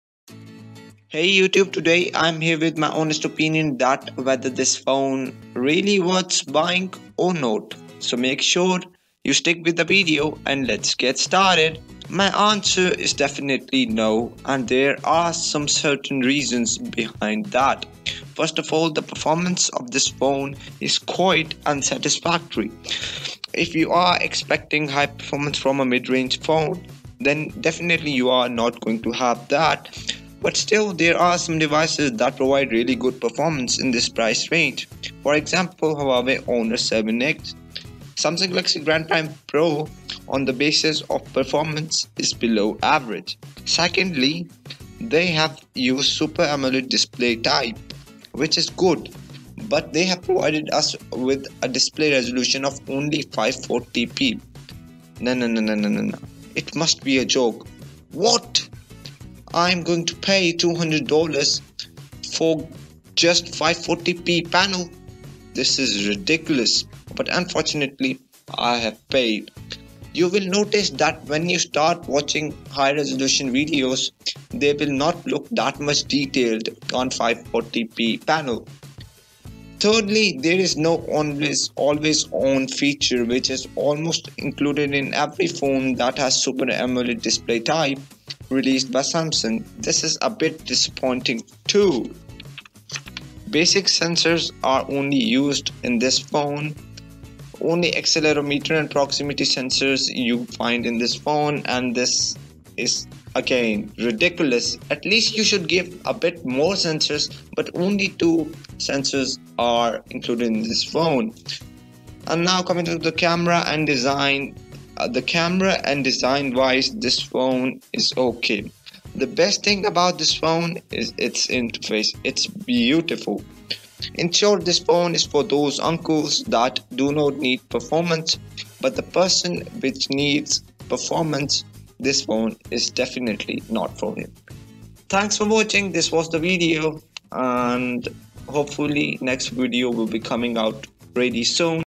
Does Samsung Galaxy Grand Prime Pro Worth Buying?
Samsung Galaxy Grand Prime Pro is a phone which was launched by "Samsung" early this year and in this video, I discussed whether Samsung Galaxy Grand Prime Pro worths buying or Samsung Galaxy Grand Prime Pro is a piece of shit? Samsung Galaxy Grand Prime Pro to me is a joke from Samsung otherwise these specs and the price I don't think match though.

You should not buy Samsung Galaxy Grand Prime Pro and there are some certain reasons why this phone is not for you! This is my point of you maybe you should have other opinions about Samsung Galaxy Grand Prime Pro. Make sure to comment your point of view.